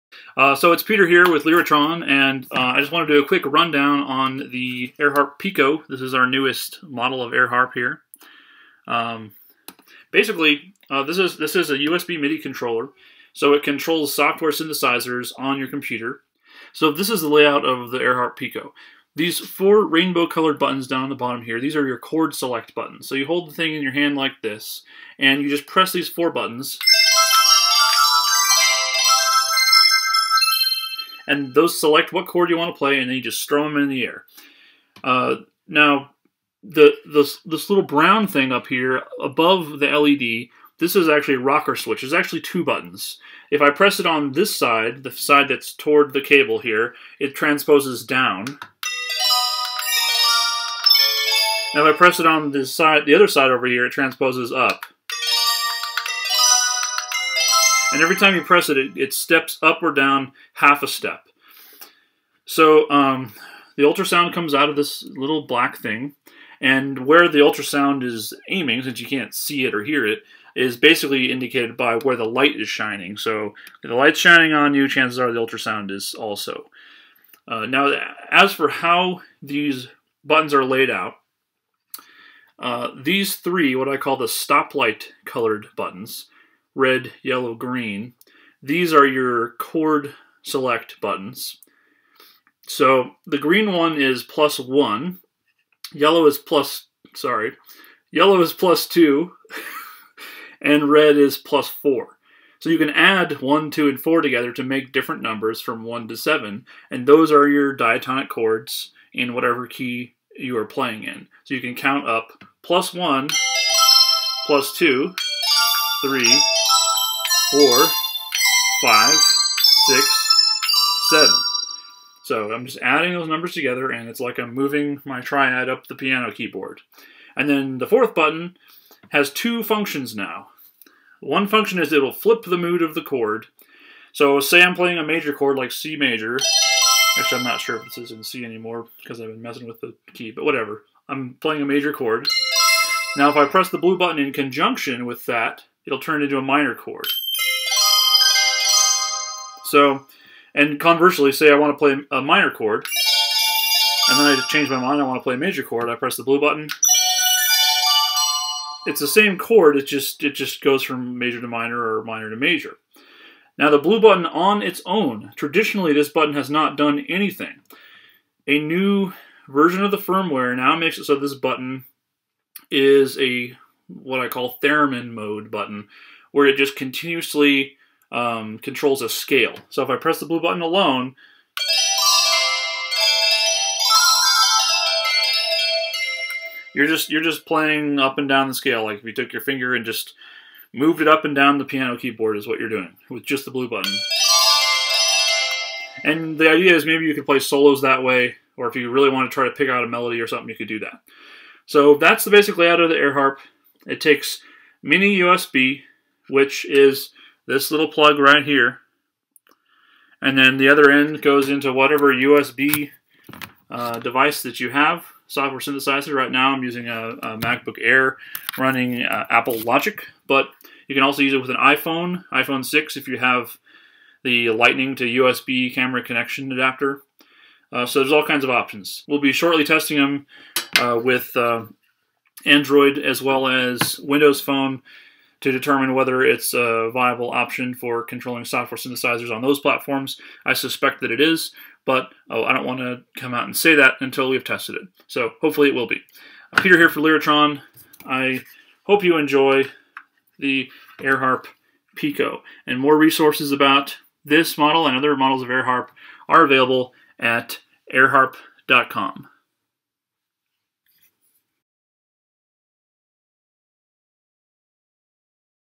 AirHarp Pico debut! (abridged)
the new AirHarp Pico works with Mac, Windows, and iPhone 6, and is available now for $100